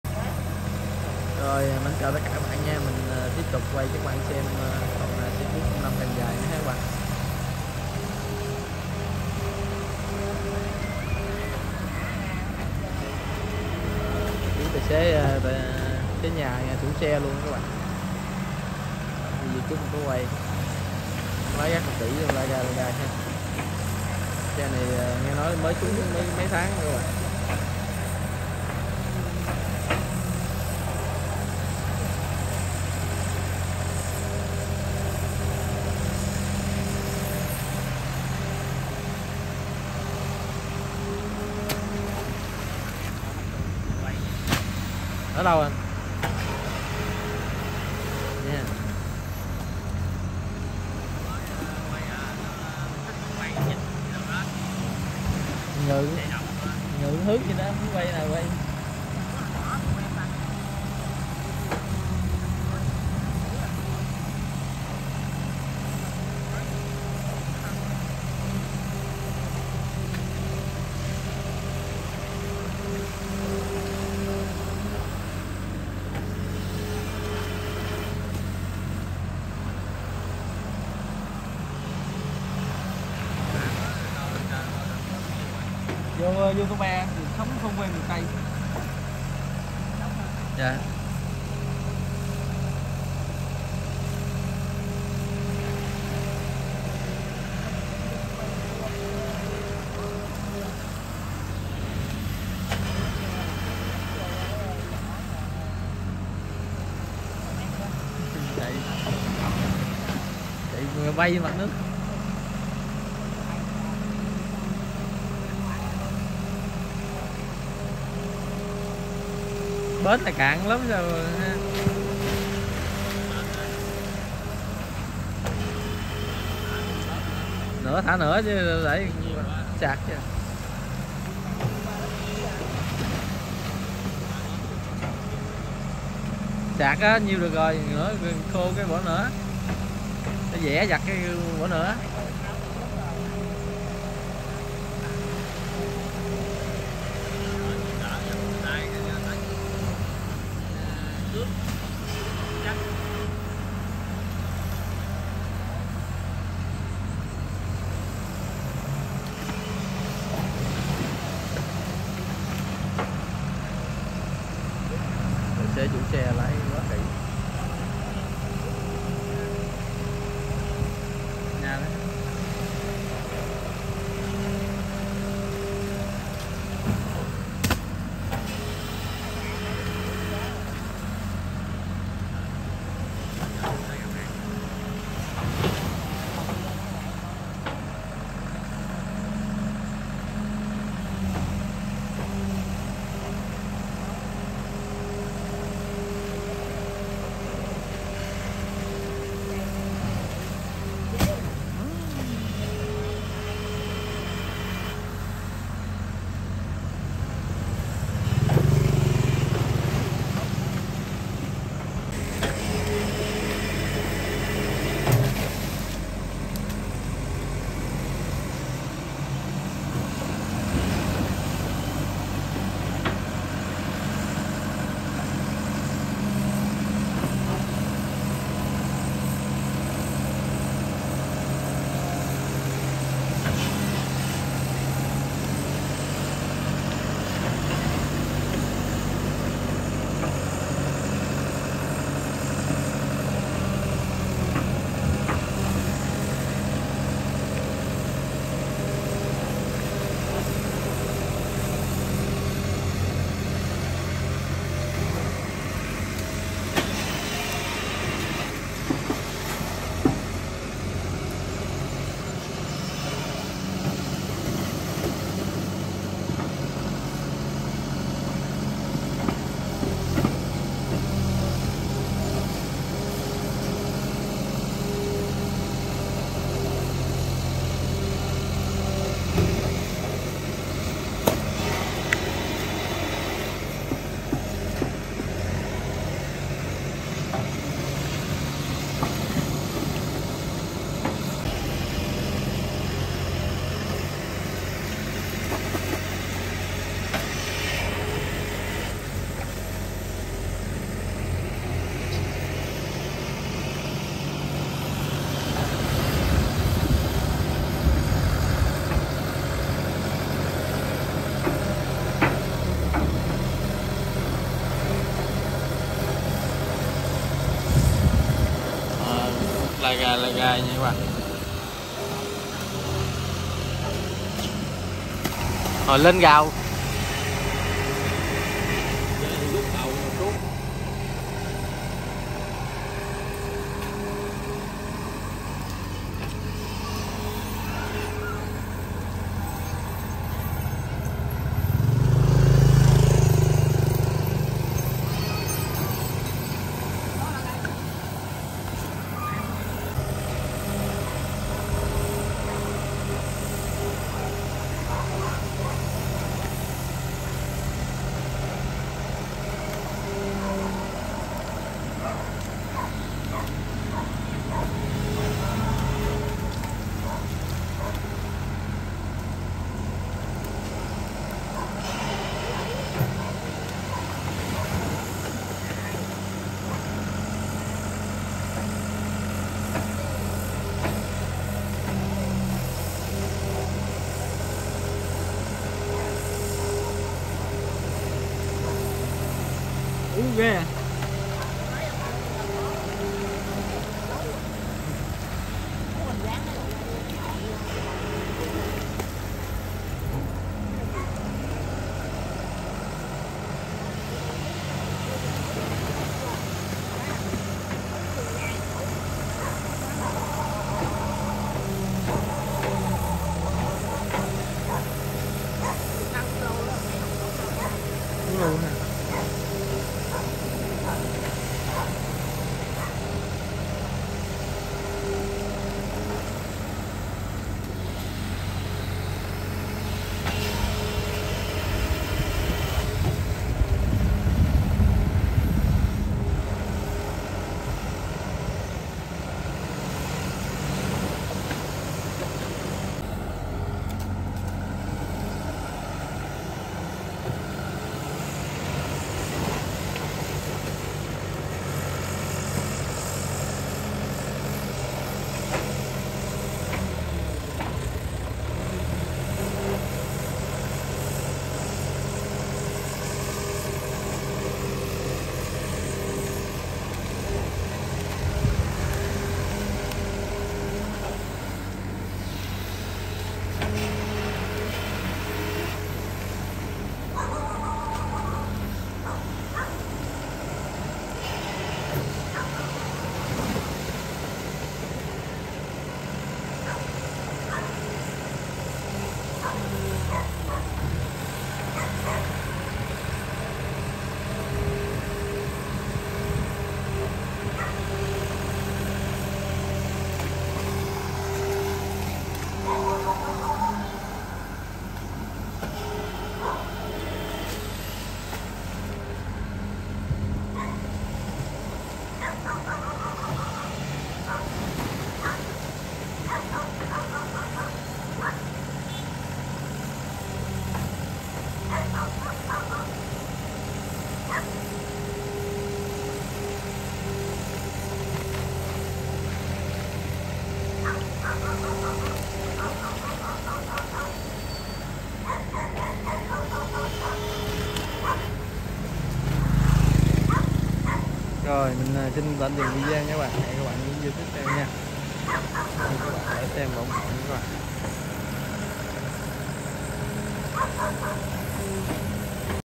tài xế chủ xe lái xe cuốc cần dài múc đất lẻ
Tài xế chủ xe lái xe cuốc cần dài múc đất lẻ bỏ vô bờ kè